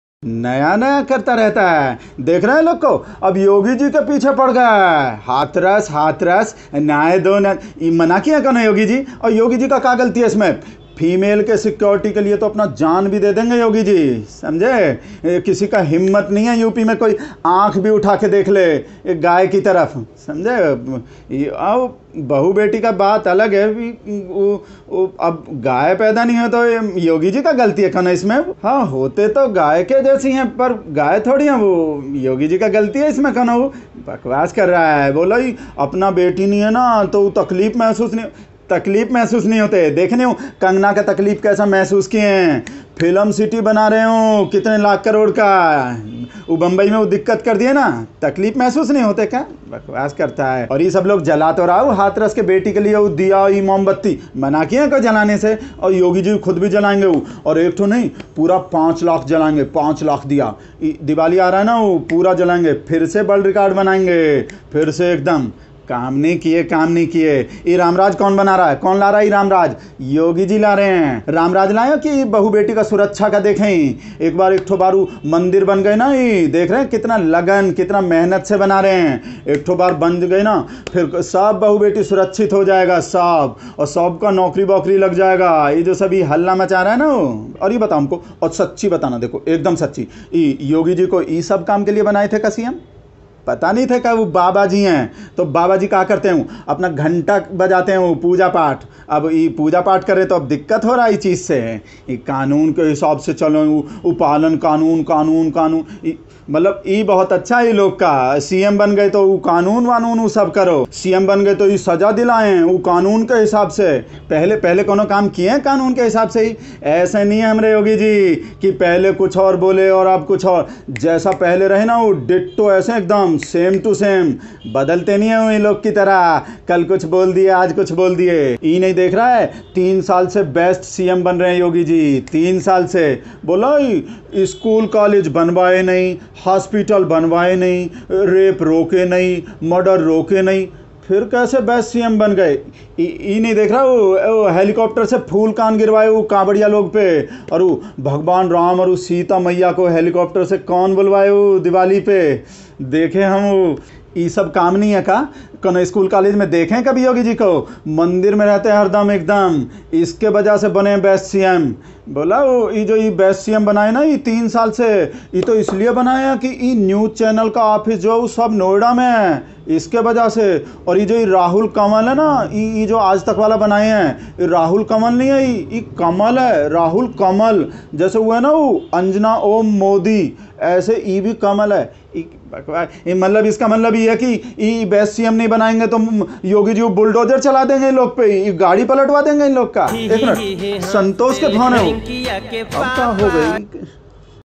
क्या आपके पास उत्तर प्रदेश में गायों को छूने की हिम्मत है? Exclusive Show
नमस्कार दोस्तो,
जब पूरा देश उत्तर प्रदेश में हो रही हालिया घटनाओं के बारे में हैरान है, तो मैं आपकी राय साझा करने के लिए कहूंगा:
यूपी कि मुख्यमंत्री श्री योगी आदित्यनाथ ji को भारत के सर्वश्रेष्ठ CM के रूप में सम्मानित क्यों नहीं किया जाना चाहिए ??

audio credits